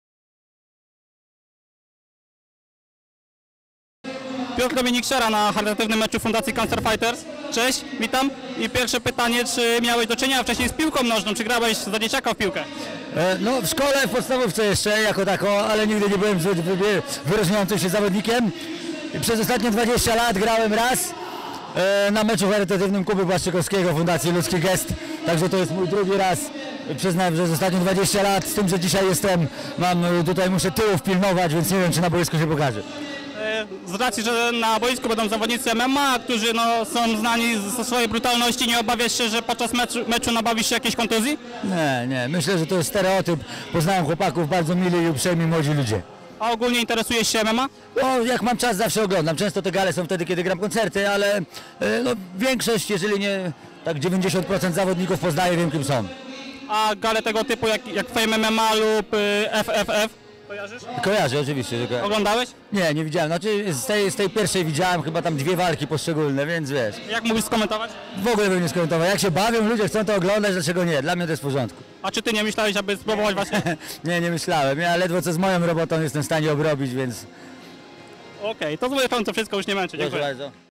Kękę o startach w MMA
Piotr Dominik Siara na meczu charytatywnym Cancer Fighters 2, gdzie porozmawialiśmy o piłce nożnej, niesieniu pomocy potrzebującym i i freakowej scenie MMA.